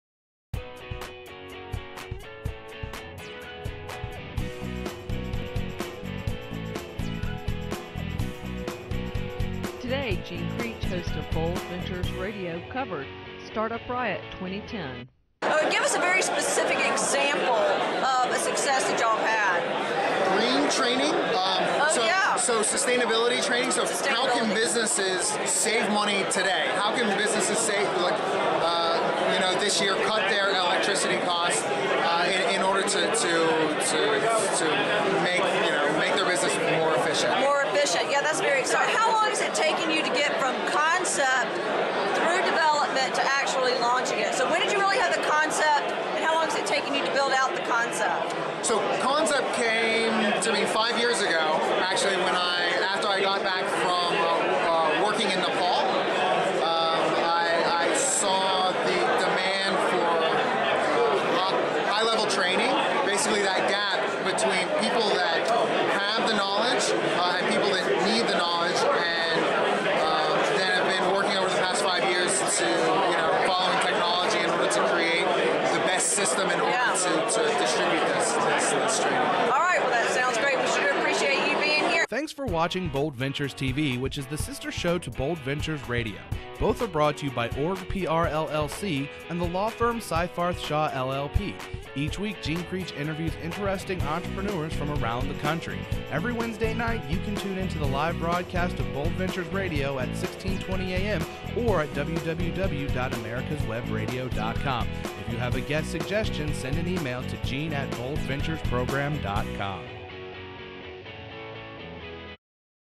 BoldVenturesTV & Learn It Live @ Startup Riot 2010
February 17, 2010.  Sid Slover talks about "green training" and how long it took to get from concept to company.  Jean Creech interviewing.